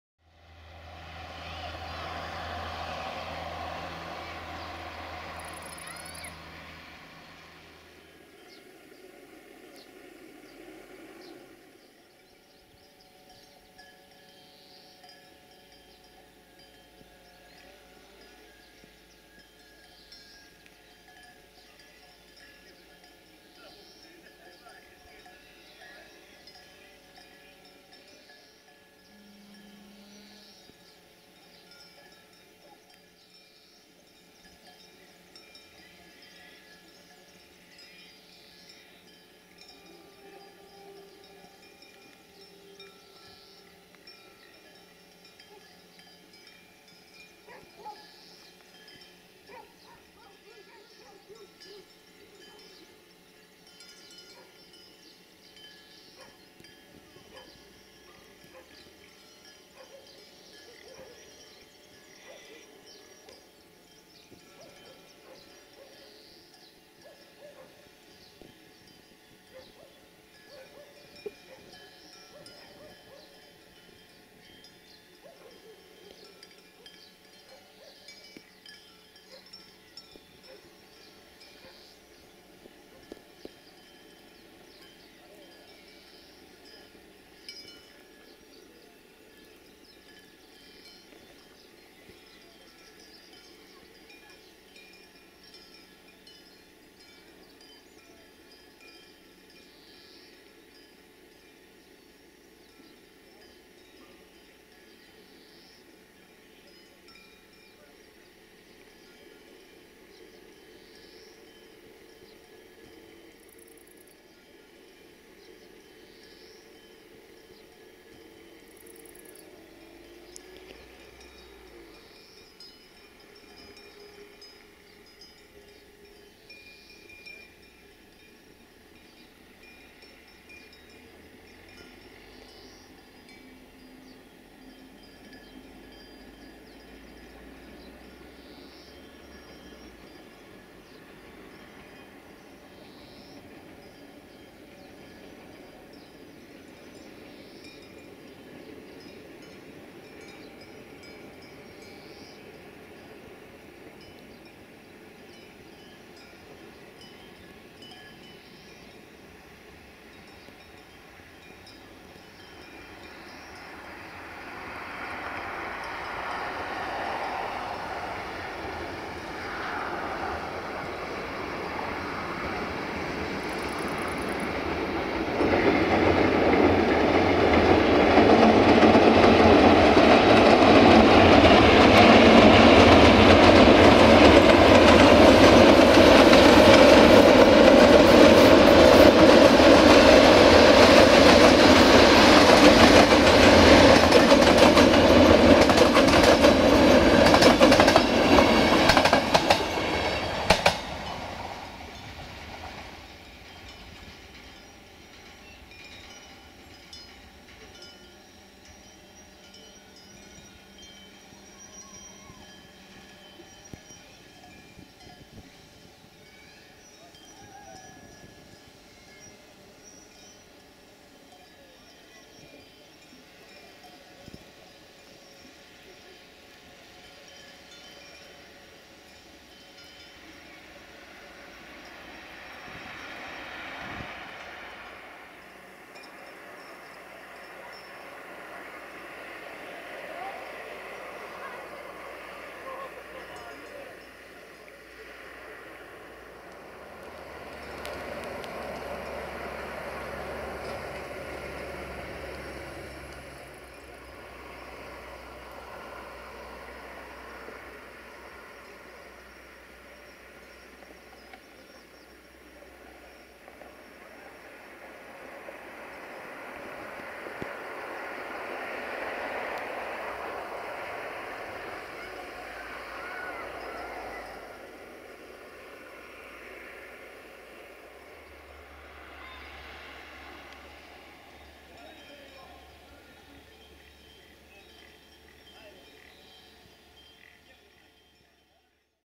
[Drone] 060-DA1 62-1051-2&Trenul Regio Express 4136 pe/on Viaduct in Dealul Stefanitei 24 July 2016
1:RE4136 - Sighetu Marmatiei - Beclean pe Somes/Bucuresti Nord/Timisoara Nord - 060-DA/ST43 - LDE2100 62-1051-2 RO-SNTFC

24 July 2016